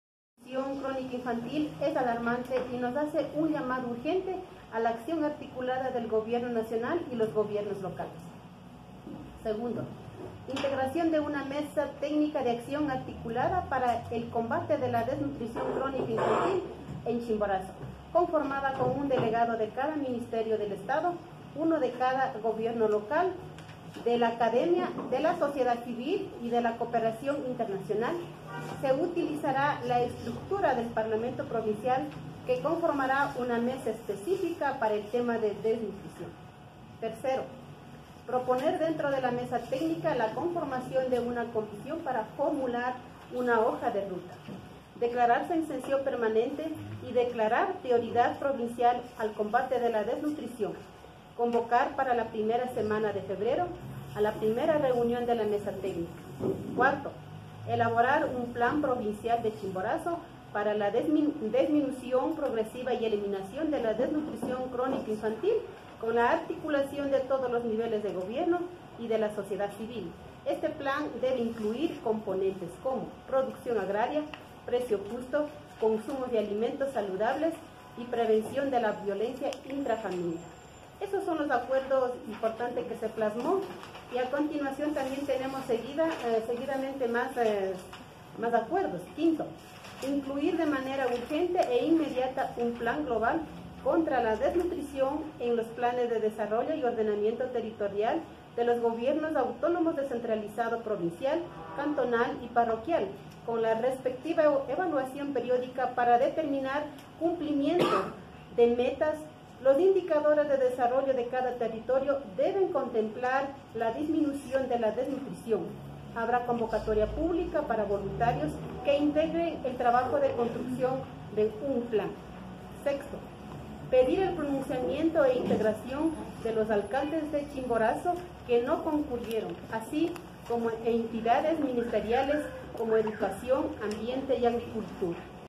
6 de febrero de 2020
Martha Chucho, técnica del Gobierno Provincial de Chimborazo, da lectura a los acuerdos llegados en el foro "Desnutrición cero". Uno de ellos la conformación de una mesa técnica para luchar contra la desnutrición. Precisamente, este acuerdo se cumple.